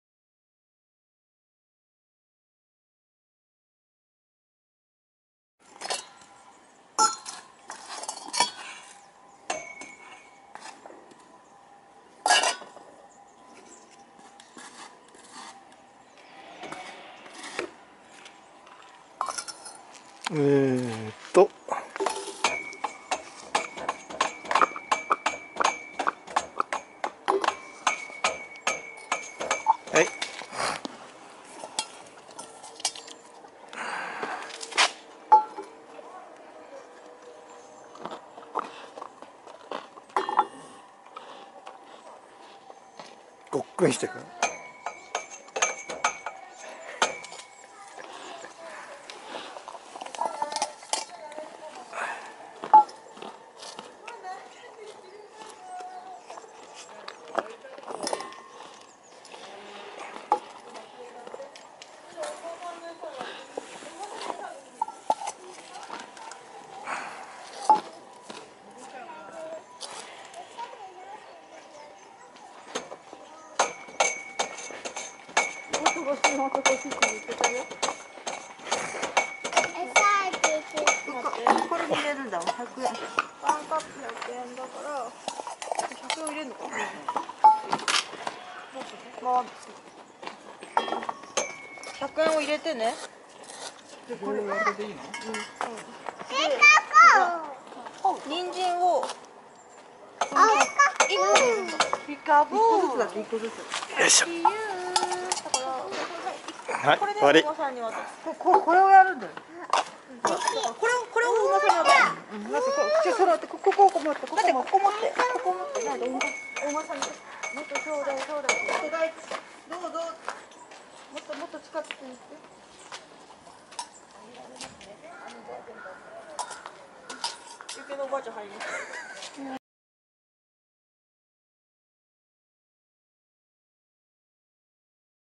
20210309 八幡山公園のポニー＆ニンジン
宇都宮市の八幡山公園の動物舎にはポニーがいます。
おやつ時に行くと￥１００でポニーへ餌やりが出来ます。
この日はおやつが売れ残っていたので、私も１回、ニンジンをあげてみました。
私の餌やりに興味を持ったのか？子供さん連れの家族が来て、最後のおやつを買ってあげていました。